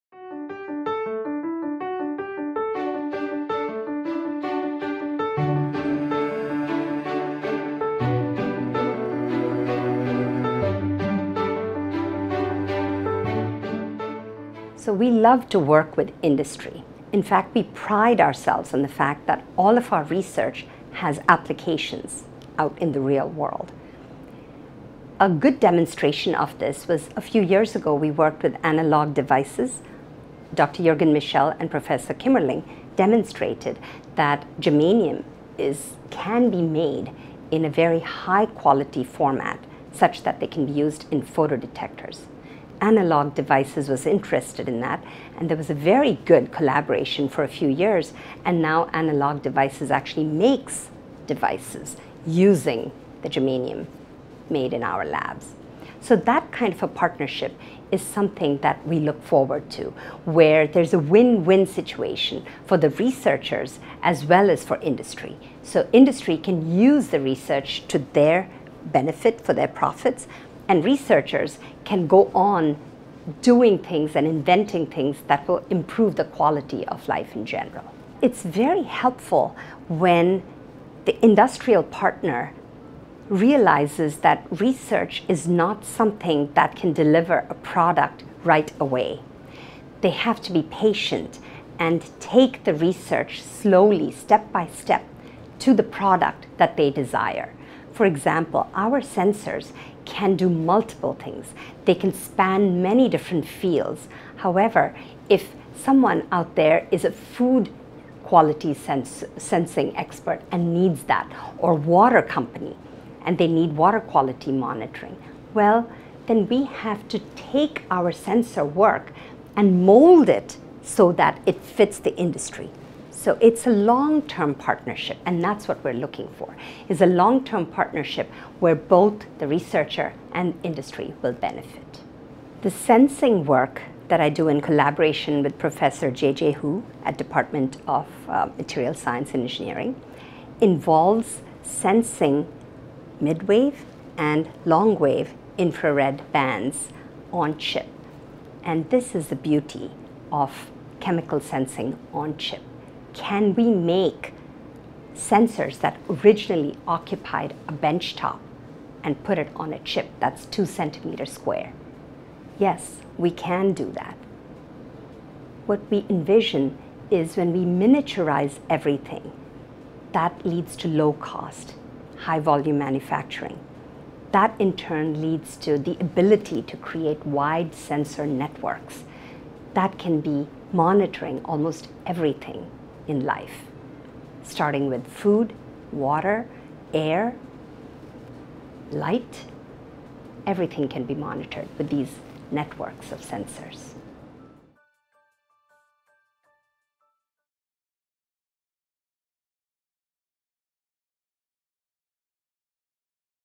Anuradha Agarwal - Industrial Partnership is Mutually Beneficial
MIT Microphotonics Center Principal Research Scientist Anuradha Agarwal pursues industrial collaboration for "win, win" outcomes.